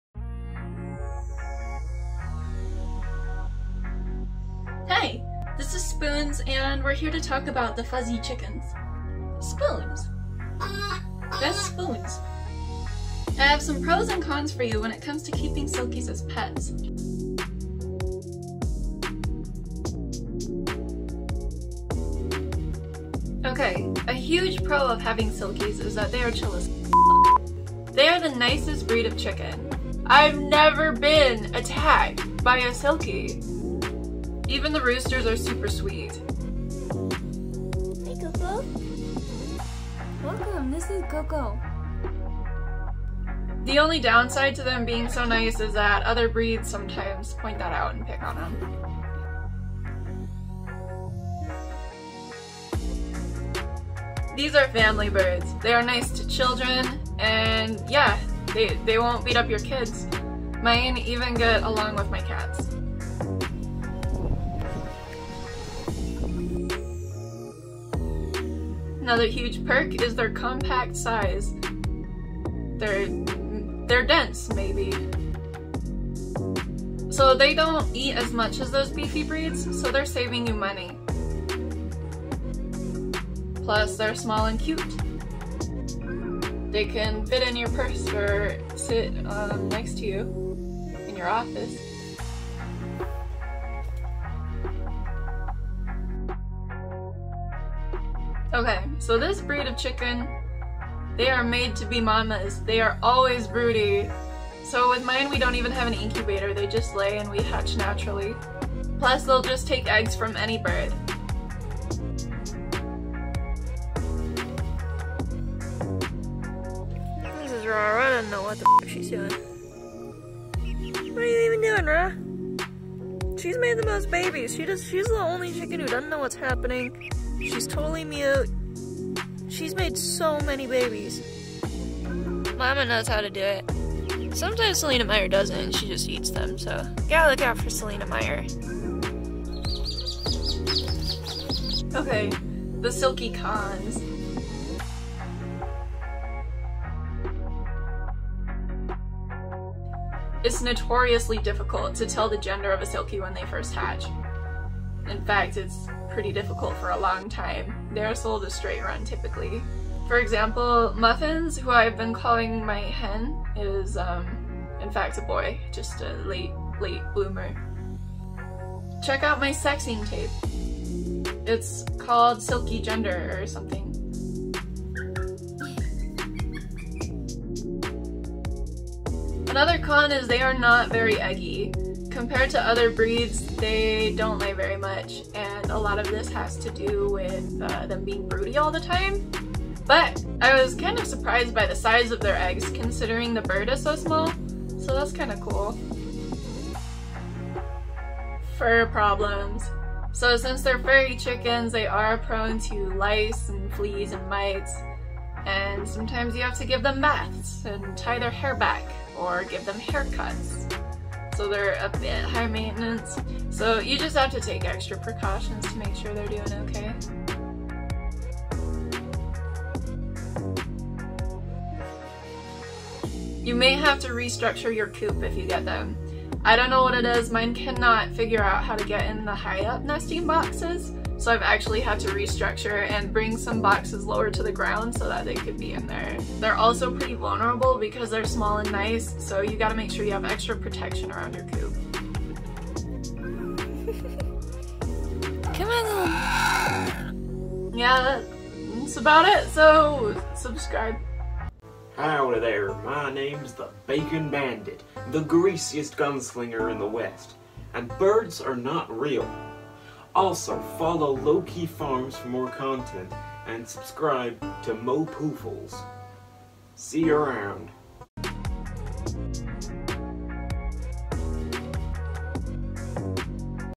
All About Silkies as Pets | Pros & Cons
All about the Silkie chicken breed. We'll cover size, fur maintenance, personality/temperament, sexing and more. Fuzzy chickens just need a little extra care